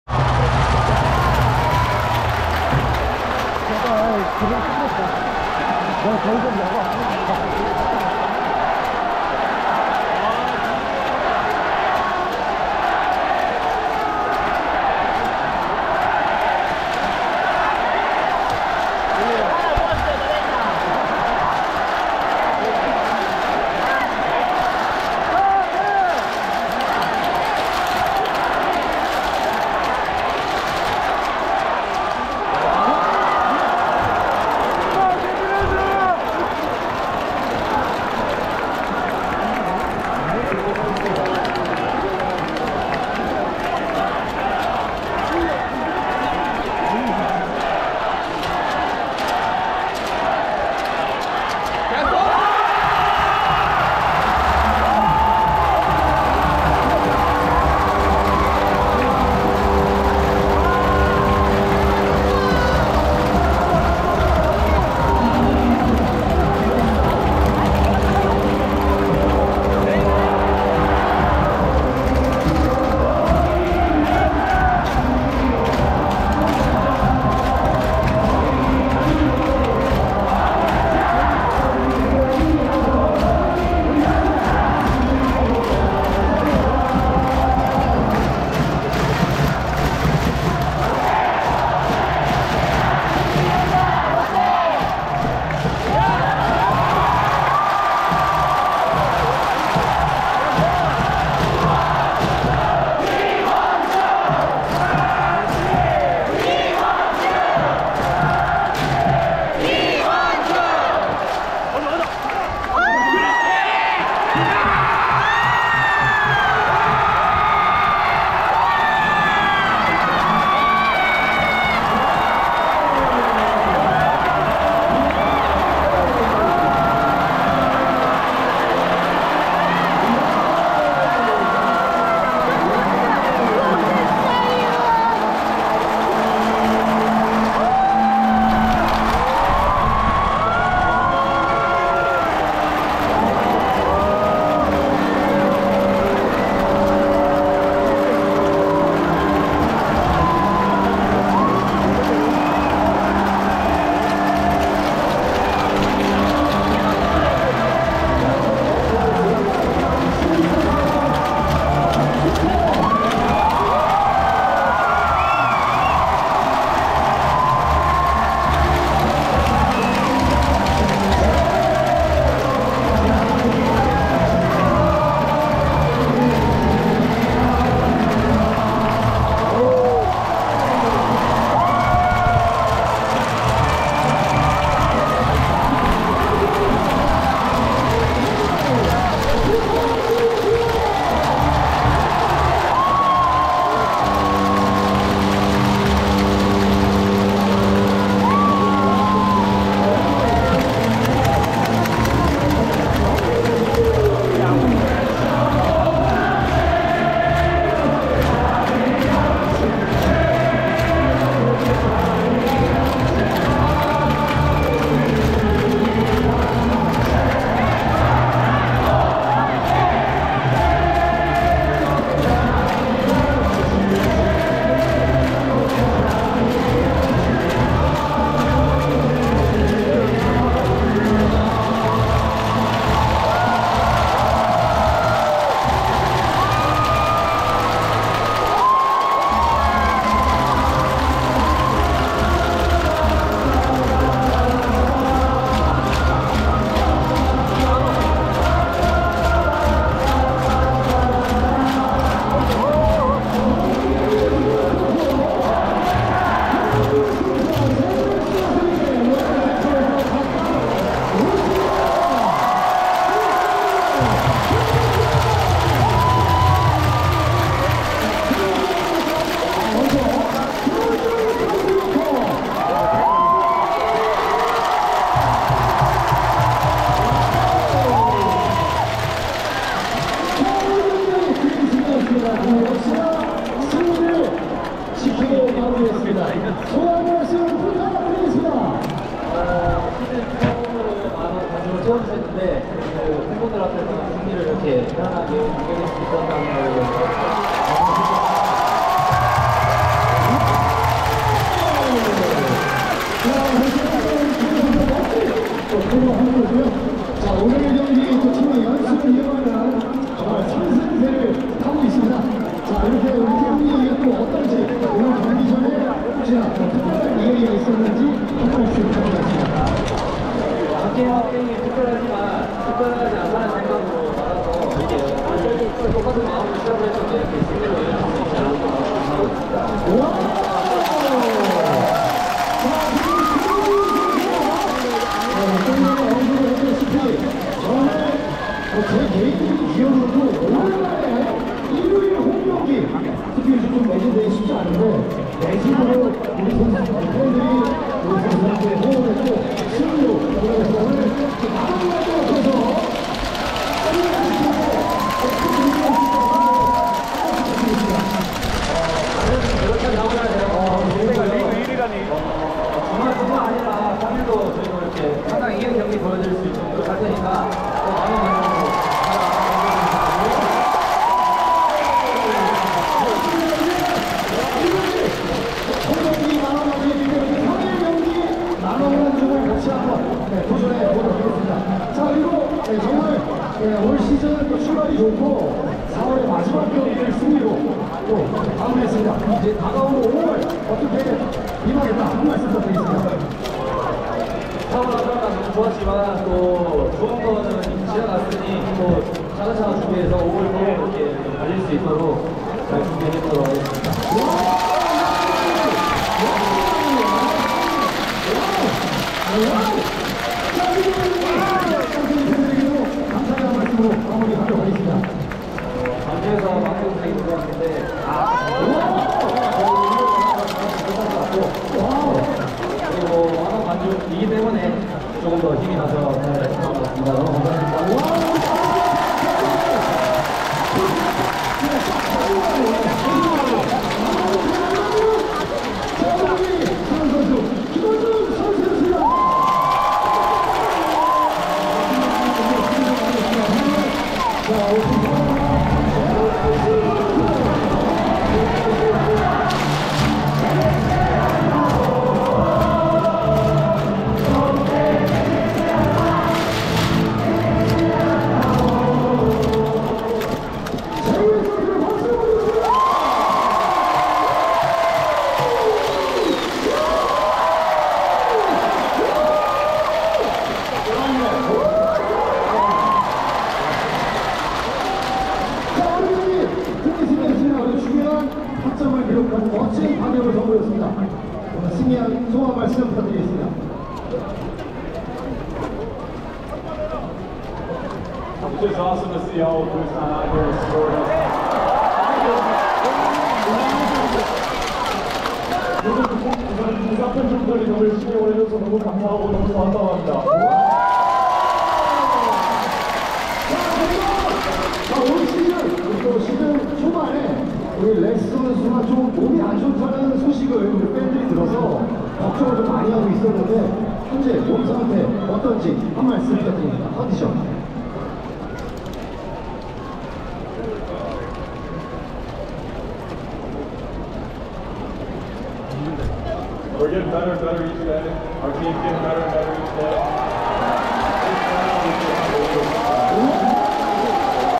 최강롯데 자이언츠 응원 무편집#8연승#사직4월의 함성 부산대한민국🇰🇷
여행 같은 일상
야구장 체험 영상입니다.
끝까지 응원해 주시면 더 좋아요
오늘도  승리
daily life like travel
This is a baseball field experience video.
please support me better
Victory today ㅇㅇㅇㅇㅇㅇㅇㅇㅇㅇㅇㅇㅇㅇ